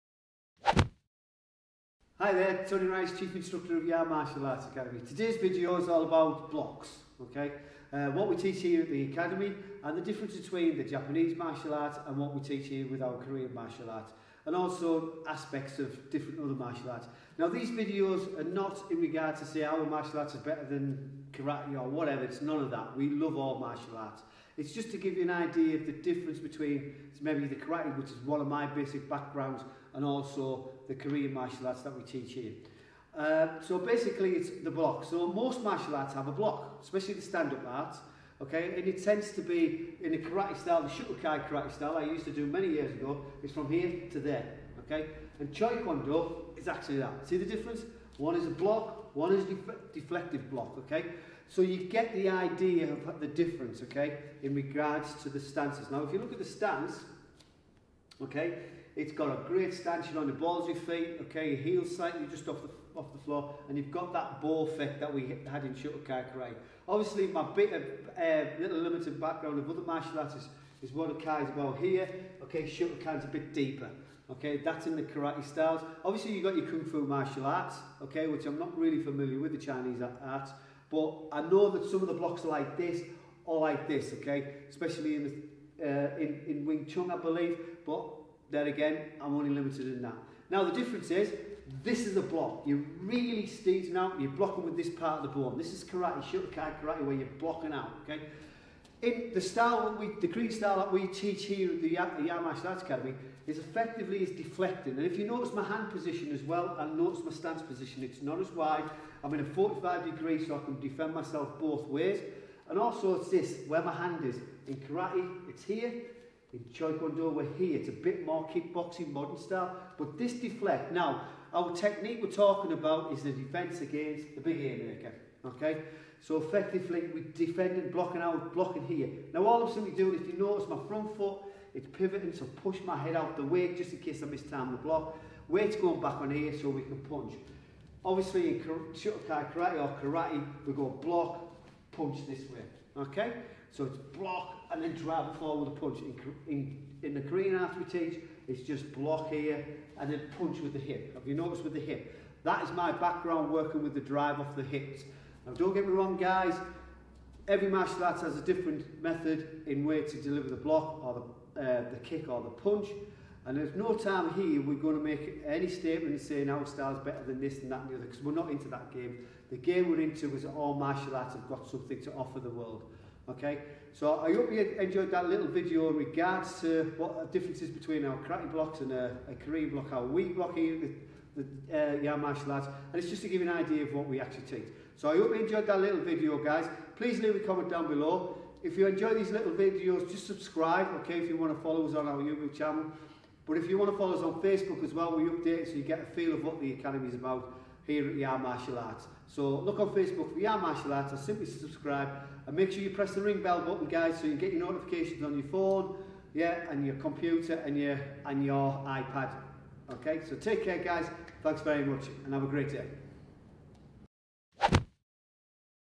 Different Styles of Blocking in Martial Arts
Hello and welcome to Yarm Martial Arts Youtube channel.

Today chief instructor Tony Rice will be going through the different styles of blocking in martial arts, looking at what we do within the academy compared to other techniques.